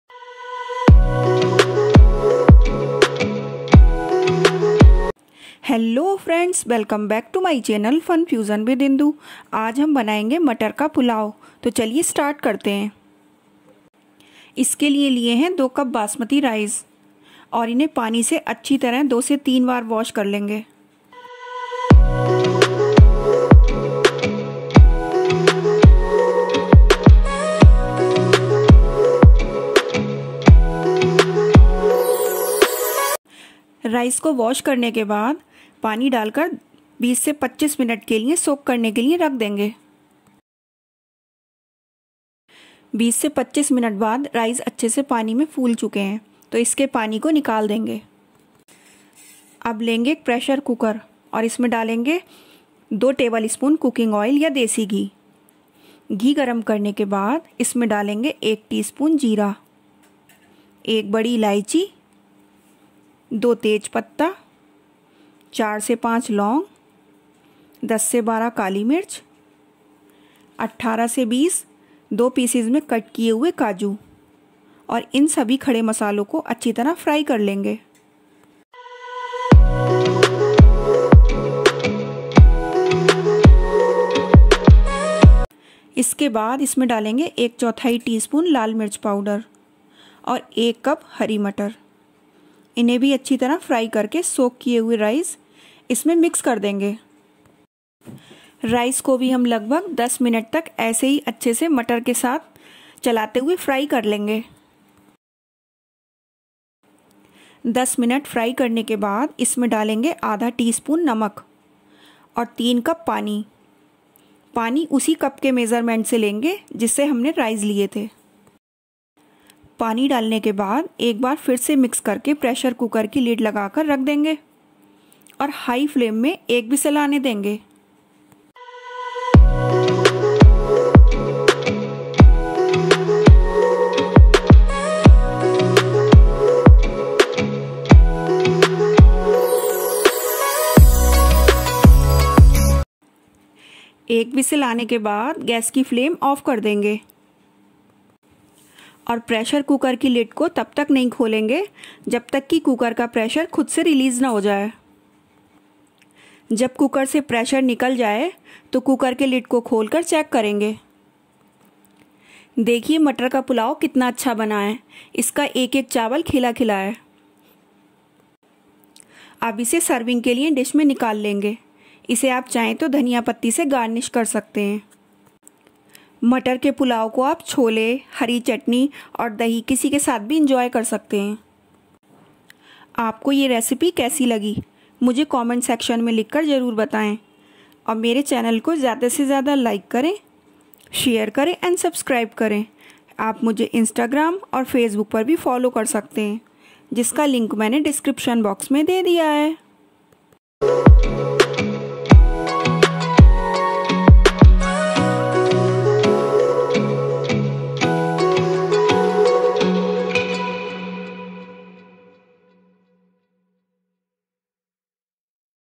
Matar Pulao | Tasty, easy and all time favourite recipe
To make this recipe ingredients required are :
• 2 cup basmati rice
• 2 tablespoon oil
• asafoetida
• cumin seeds
• cardamom
• cloves
• black pepper
• bay leaves
• cashew nuts
• red chilli powder
• salt
• 3 cup water
Till then keep watching and meet you in the next video 🙂